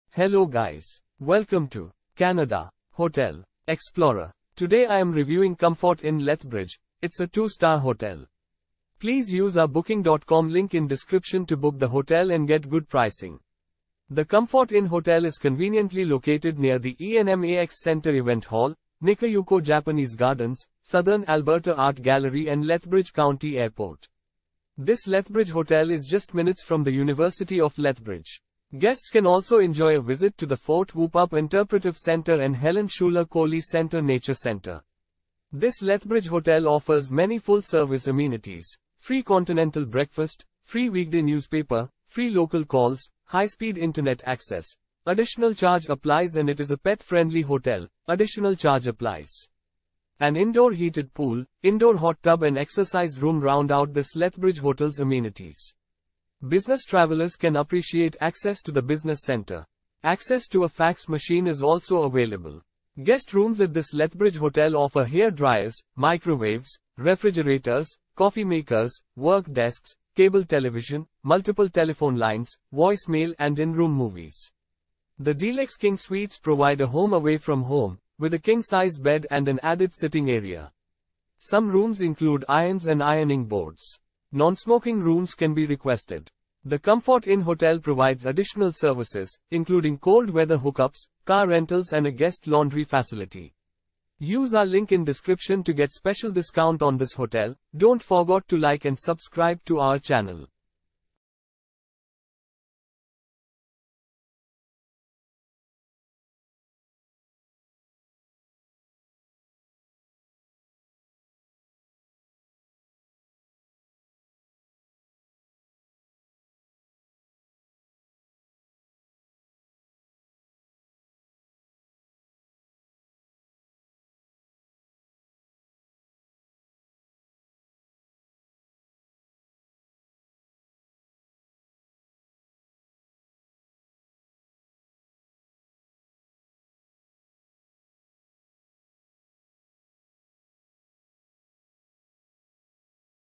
Comfort Inn Lethbridge Review - Lethbridge , Canada
Comfort Inn Lethbridge

The Comfort Inn hotel is conveniently located near the ENMAX Center event hall, Nikka Yuko Japanese Gardens, Southern Alberta Art Gallery and Lethbridge County Airport. This Lethbridge hotel is just minutes from the University of Lethbridge. Guests can also enjoy a visit to the Fort Whoop-Up Interpretive Center and Helen Schuler Coulee Center nature center.

This Lethbridge hotel offers many full-service amenities: free continental breakfast, free weekday newspaper, free local calls, high-speed Internet access; additional charge applies and it is a pet-friendly hotel; additional charge applies. An indoor heated pool, indoor hot tub and exercise room round out this Lethbridge hotel's amenities. Business travelers can appreciate access to the business center. Access to a fax machine is also available.

Guest rooms at this Lethbridge hotel offer hair dryers, microwaves, refrigerators, coffee makers, work desks, cable television, multiple telephone lines, voice mail and in-room movies. The deluxe king suites provide a home away from home, with a king-size bed and an added sitting area. Some rooms include irons and ironing boards. Nonsmoking rooms can be requested.

The Comfort Inn hotel provides additional services, including cold weather hookups, car rentals and a guest laundry facility.